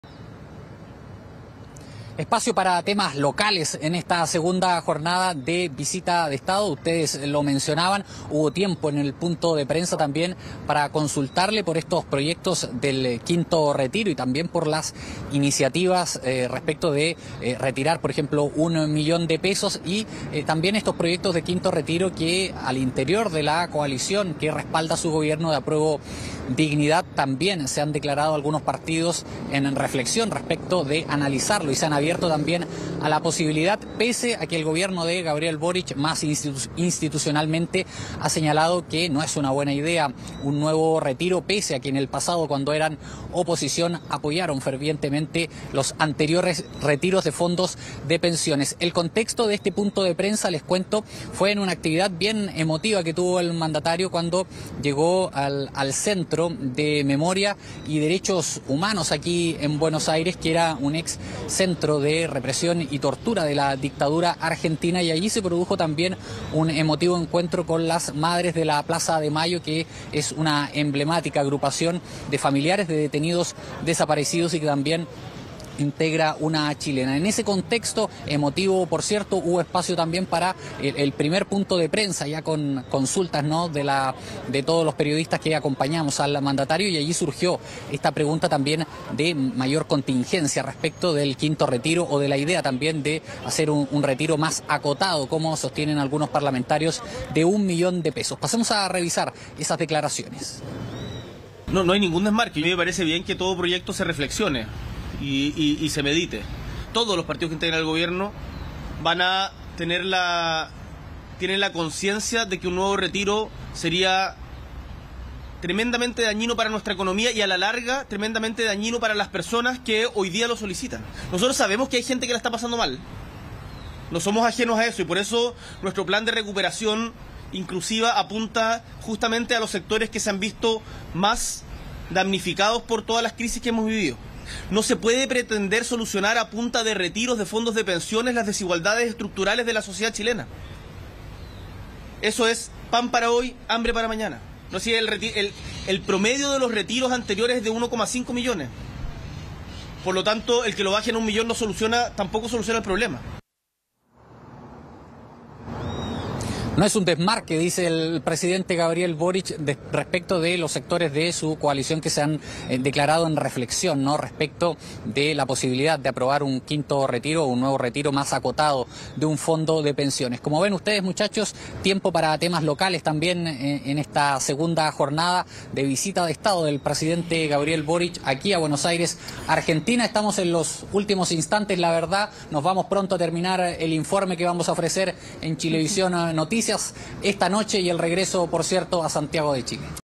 Pdte. Boric REITERA SU RECHAZO al quinto retiro: “Eso es pan para hoy y hambre para mañana”
El mandatario indicó que, aunque sea una medida “impopular”, su administración y los partidos del Gobierno no apoyarán iniciativas de este tipo.